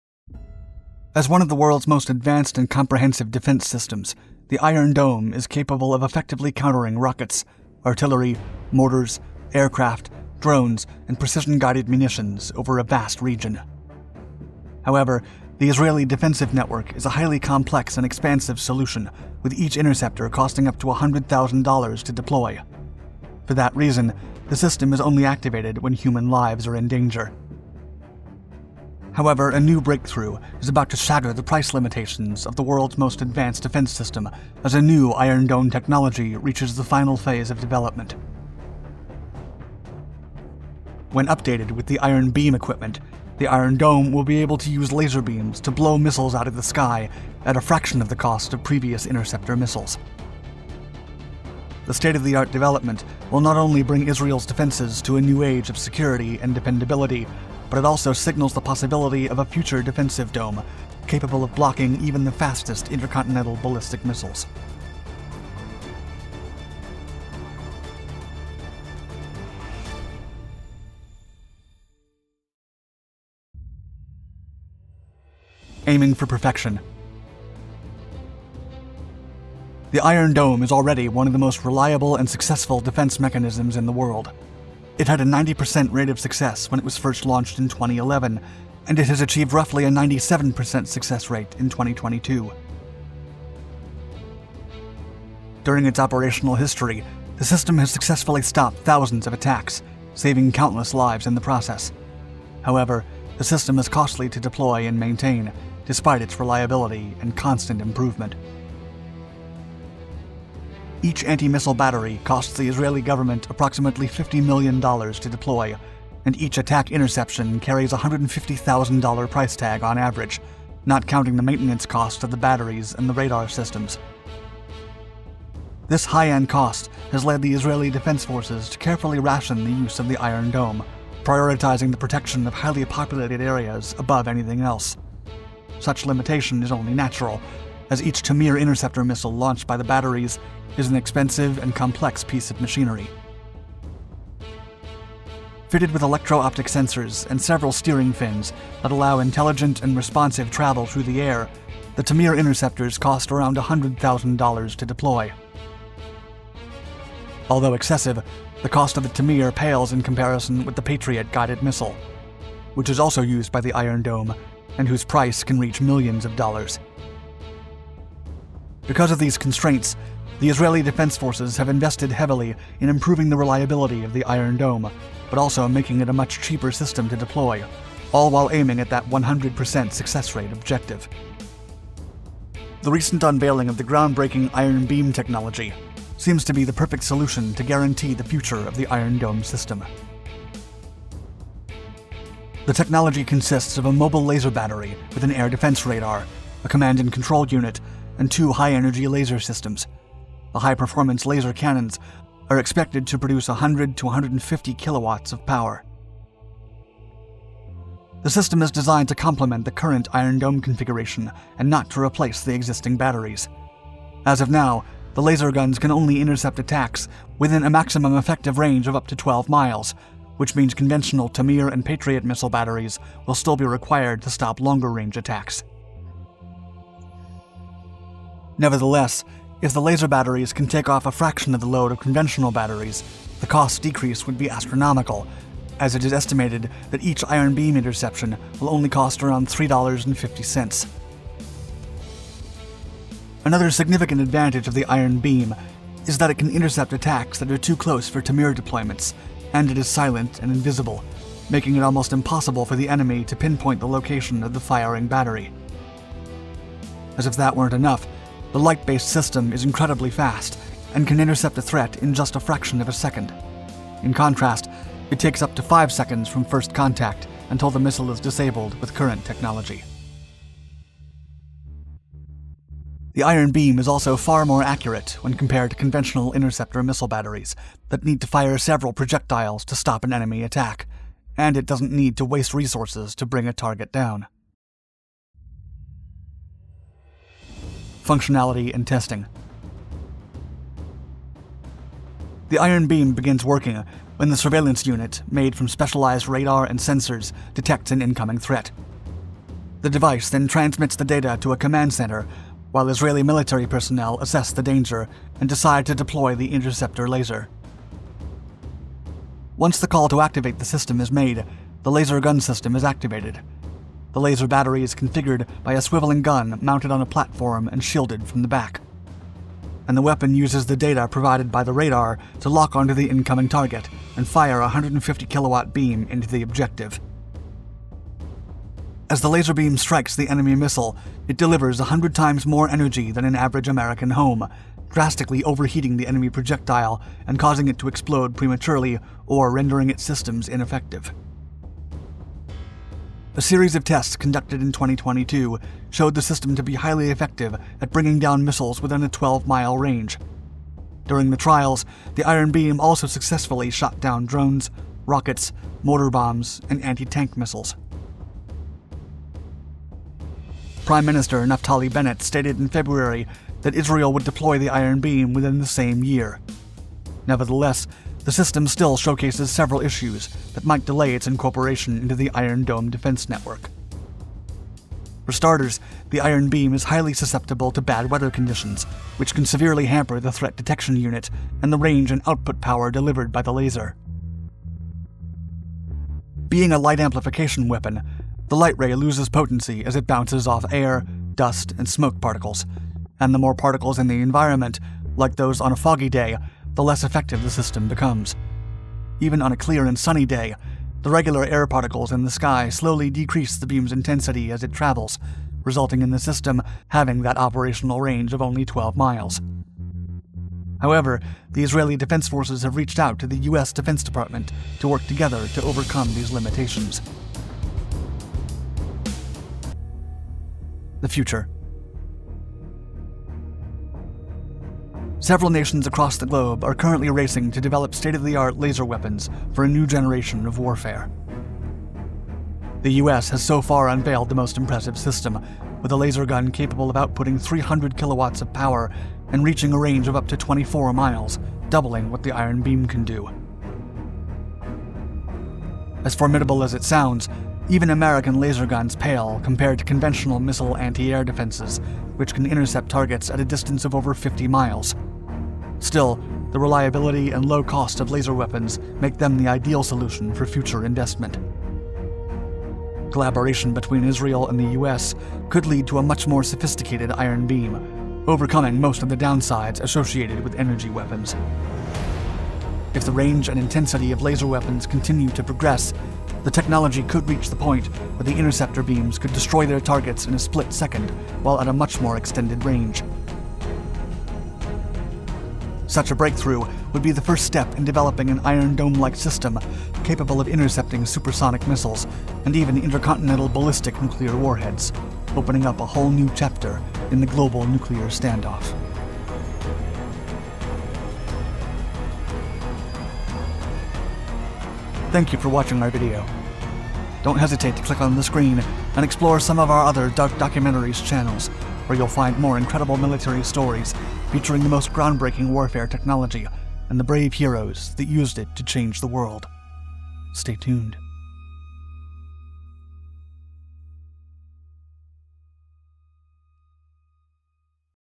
The Iron Beam That Makes Israel an Impenetrable Country
As one of the world’s most advanced and comprehensive defense systems, the Iron Dome is capable of effectively countering rockets, artillery, mortars, aircraft, drones, and precision-guided munitions over a vast region.

However, the Israeli defensive network is a highly complex and expensive solution, with each interceptor costing up to 100,000 dollars to deploy. For that reason, the system is only activated when human lives are in danger.

A new breakthrough, however, is about to shatter the price limitations of the world’s most advanced defense system as a new ‘Iron Dome’ technology reaches the final phase of development.

When updated with the Iron Beam equipment, the Iron Dome will be able to use laser beams to blow missiles out of the sky at a fraction of the cost of previous interceptor missiles.

The state-of-the-art development will not only bring Israel’s defenses to a new age of security and dependability, but it also signals the possibility of a future defensive dome capable of blocking even the fastest intercontinental ballistic missiles…